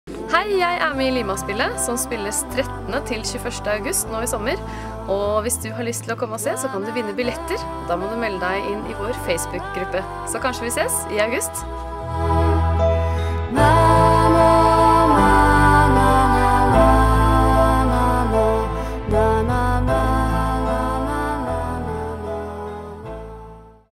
Øyunn.mov
Limaspillet 2010 med bl.a. Gaute Ormåsen, Øyunn Bjørge, Elin Marie Nordeng og Jorun MH Skryseth. Spilles fra 13. - 21. august på badeplassen Lima ved sørenden av Hurdalsjøen. Billetter i salg nå hos Billettservice! Les mer på ww.limaspillet.no